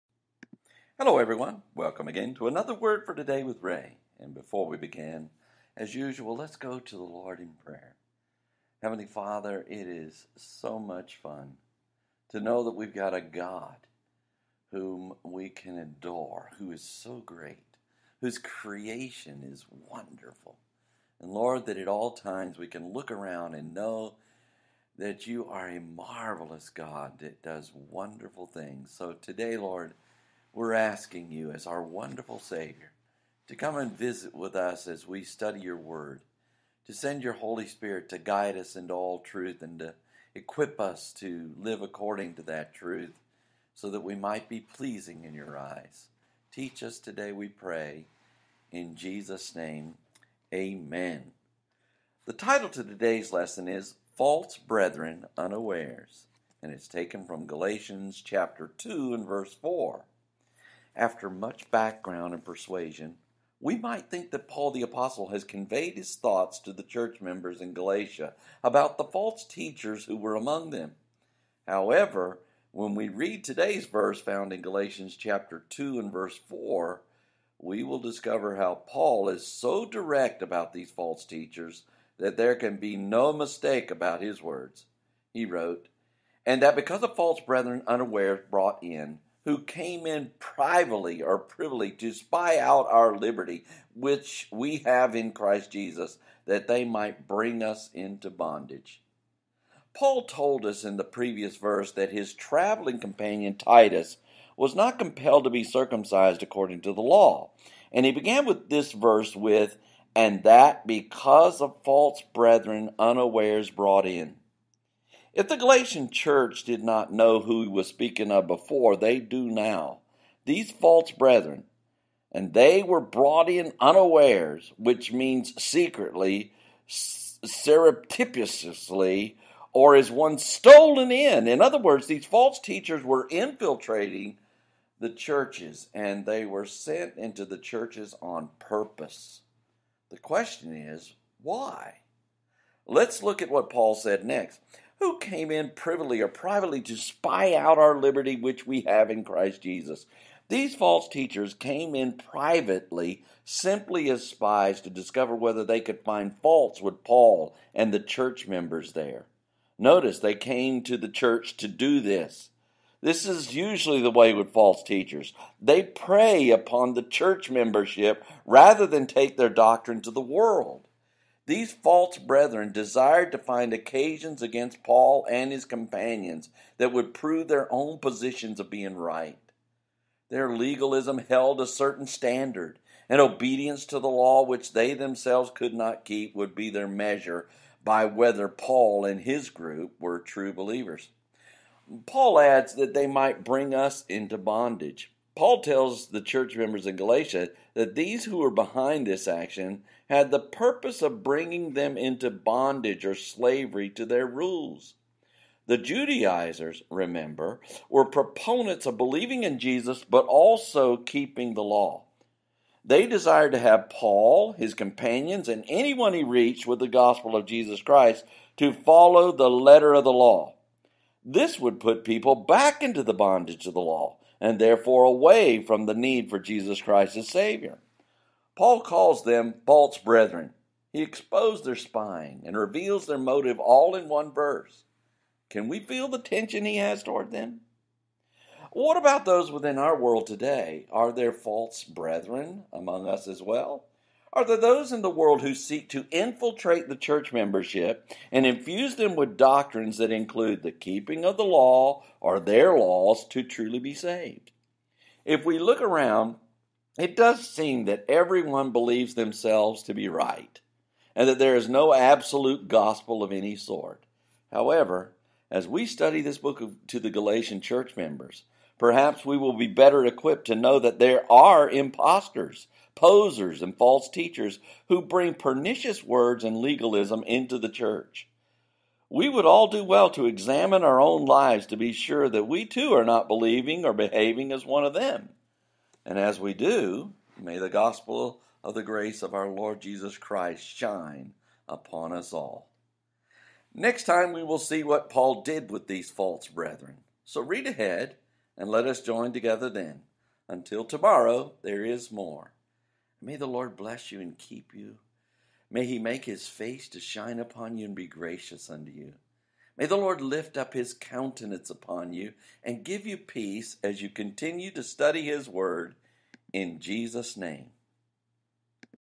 False Brethren Unawares - Galatians 2:4 (made with Spreaker)
Verse by verse study through the book of Galatians Chapter Two and Verse Four